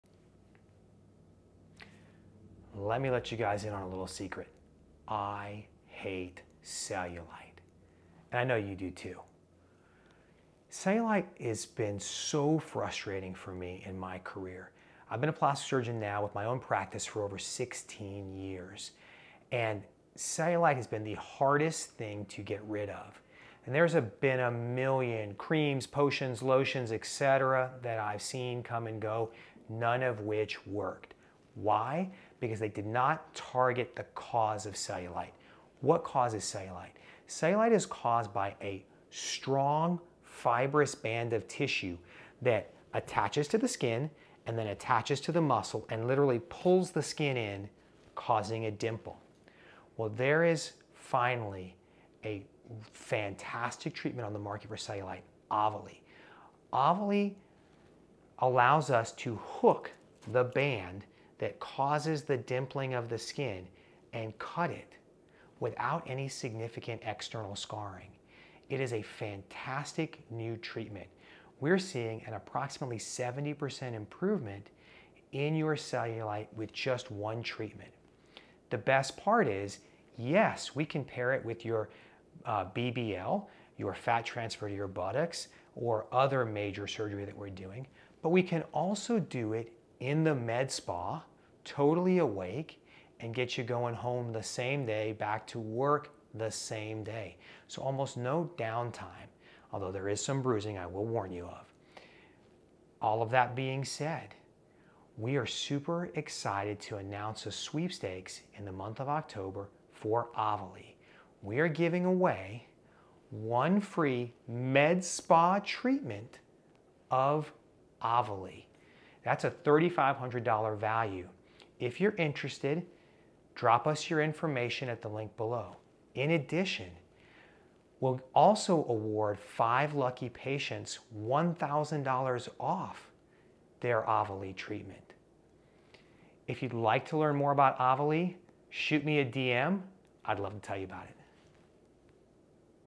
Get Rid of Cellulite with Aveli + Sweepstakes!!
In the month of October we are giving away 1 free medspa treatment of Aveli!! Watch the video for the details!!!